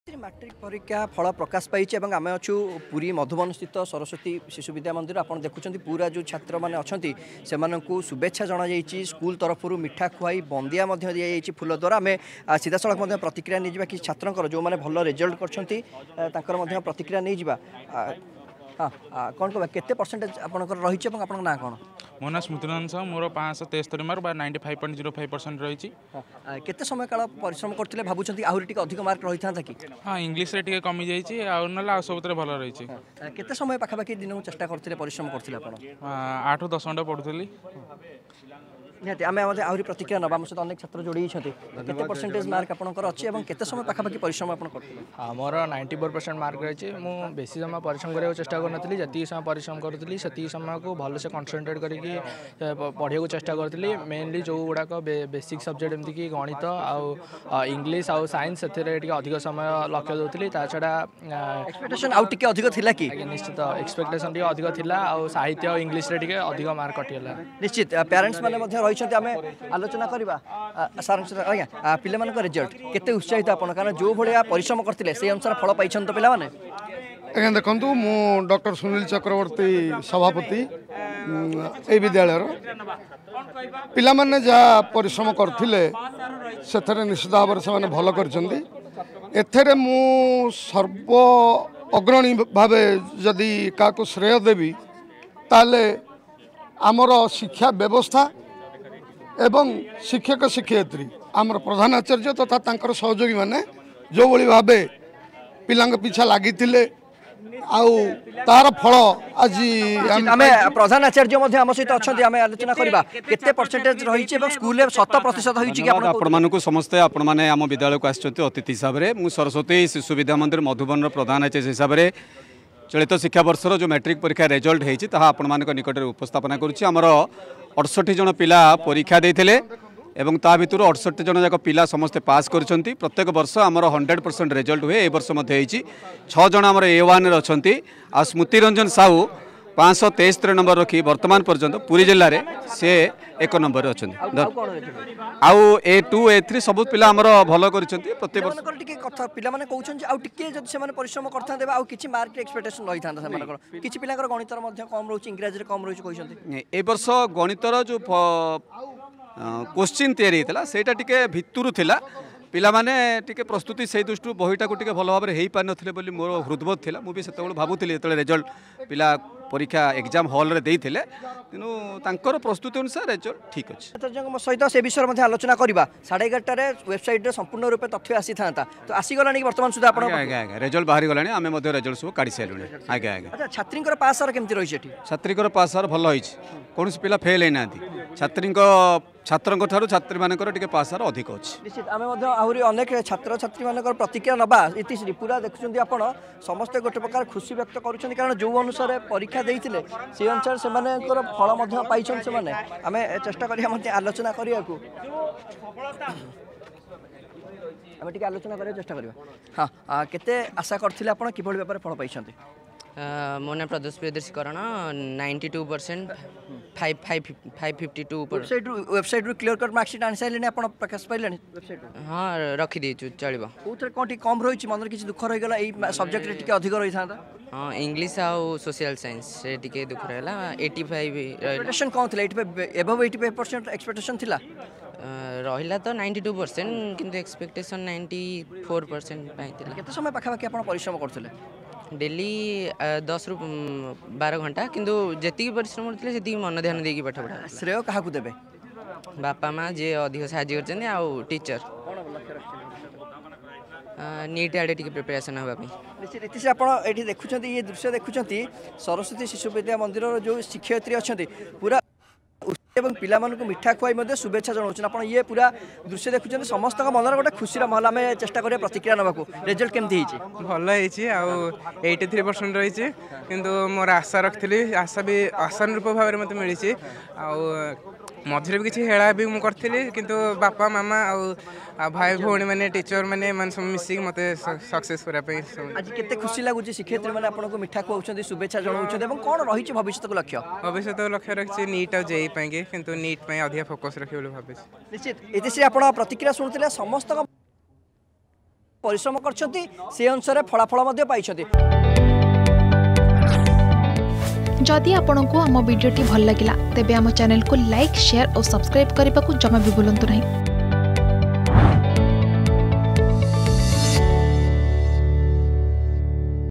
ପୁରୀ ମଧୁବନ ସରସ୍ଵତୀ ଶିଶୁ ବିଦ୍ୟା ମନ୍ଦିରରେ ମାଟ୍ରିକ୍‌ ପରୀକ୍ଷା ଫଳ ପ୍ରକାଶ । || MatricResult
ପୁରୀ ମଧୁବନ ସରସ୍ଵତୀ ଶିଶୁ ବିଦ୍ୟା ମନ୍ଦିରରେ ମାଟ୍ରିକ୍‌ ପରୀକ୍ଷା ଫଳ ପ୍ରକାଶ ।